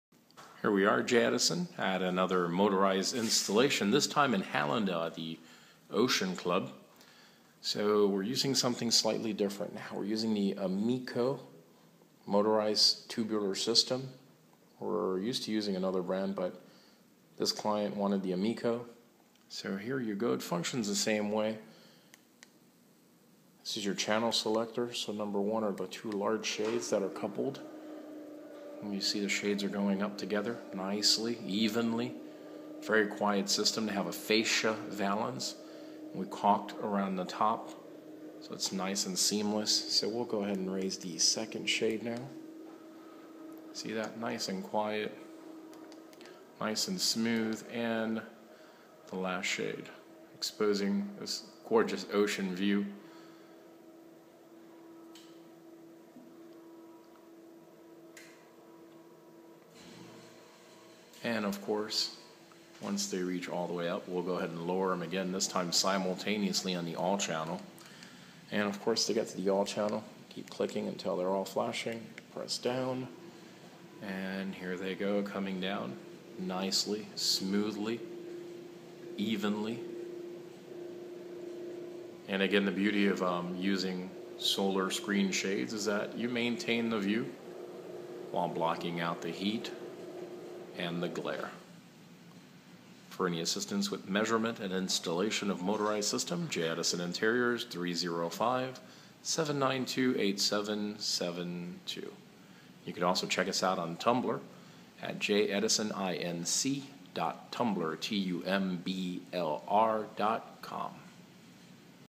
Motorized screen shades, Amico system
Motorized solar screen shades using Amico tubular motors.